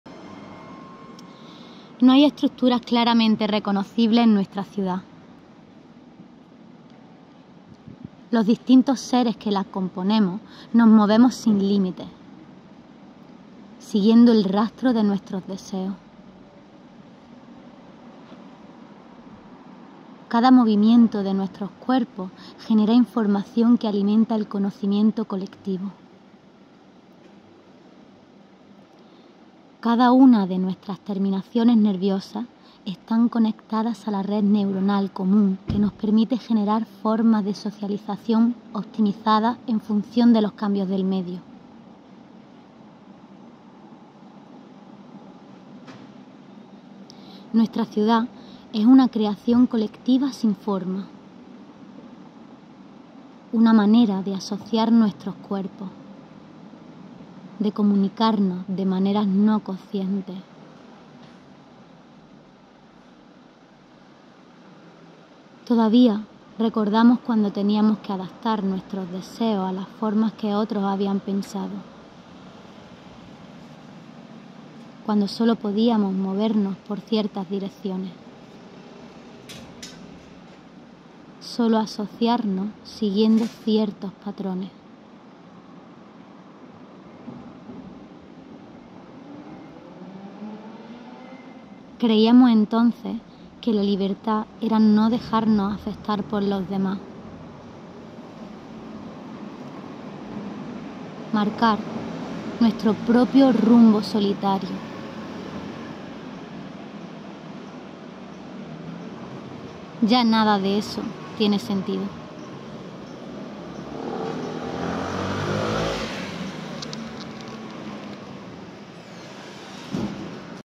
ENTREPLANOS 7: La ciudad sin forma
"No hay estructuras claramente reconocibles en nuestra ciudad. Los distintos seres que la componemos nos movemos sin límites, siguiendo el rastro de nuestros deseos. Cada movimiento de nuestros cuerpos genera información que alimenta el conocimiento colectivo. Cada una de nuestras terminaciones nerviosas estan conectadas a la red neuronal común que nos permite generar formas de socialización optimizadas en funcion de los cambios del medio. Nuestra ciudad es una creacion colectiva sin forma. Un modo de asociar nuestros cuerpos, de comunicarnos de maneras no conscientes; Todavia recordamos cuando teniamos que adaptar nuestros deseos a las formas que otros habían pensado, cuando solo podíamos movernos por ciertas direcciones. Solo asociarnos siguiendo ciertos patrones… Creiamos entonces que la libertad era no dejarnos afectar por los demás, dibujar  nuestro propio rumbo solitario. Ya nada de eso tiene sentido".

[Propuesta presentada al certamen de vídeos con el móvil Entreplanos7. Descripción aportada por sus autores/as]